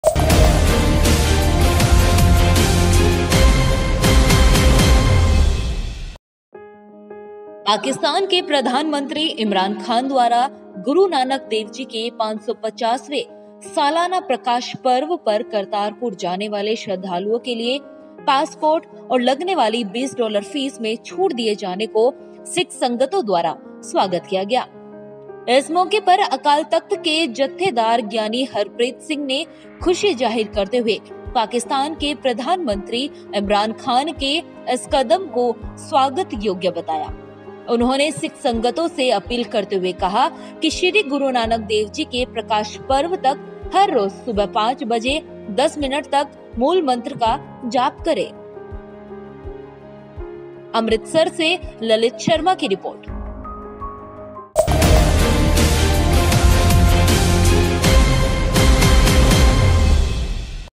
सिख संगतों ने जताई ख़ुशी...
पाकिस्तान के प्रधानमंत्री इमरान खान द्वारा गुरु नानक देव जी के 550 वें सालाना प्रकाशपर्व पर करतारपुर जाने वाले श्रधालुओं के लिए पासपोर्ट और लगने वाली 20 डॉलर फीस में छुट दिए जाने को सिख संगतों द्वारा स्वागत किया गया...इस मौके पर अकाल तख़्त के जत्थेदार ज्ञानी हरप्रीत सिंह ने ख़ुशी जाहिर करते हुए पाकिस्तान के प्रधानमंत्री इमरान खान के इस कदम को स्वागत योग्य बताया...उन्होंने सिख संगतों से अपील करते हुए कहा है कि श्री गुरु नानक देव जी के प्रकाशपर्व तक हर रोज सुबह 5 बजे 10 मिनट तक मूलमंत्र का जाप करें...!